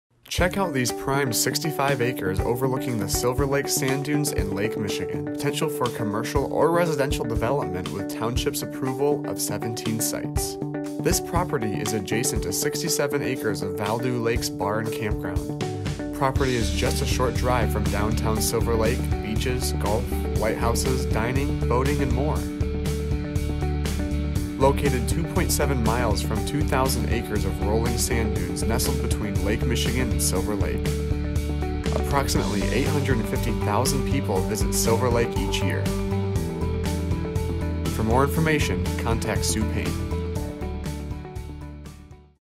65+/- Acres Wilson Road, Mears, MI 49436
Prime 65 acres overlooking the Silver Lake Sand Dunes and Lake Michigan! Approximately half of this property is serviced with a public paved road leading to privately paved roads accessing this world class view. The potential for this property will excite any would-be investor willing to finish this development with utilities and subdivision restrictions or this would make an interesting location for a possible golf course, or other such development with township approval. This property is next to 67 Acre Val-Du Lakes Bar & Campground and a few miles from the popular Silver Lake Sand Dunes ORV Area and Lake Michigan. The remaining acreage for future planning has public roads on 3 sides. Includes 600 Ft of Hunters Creek Frontage.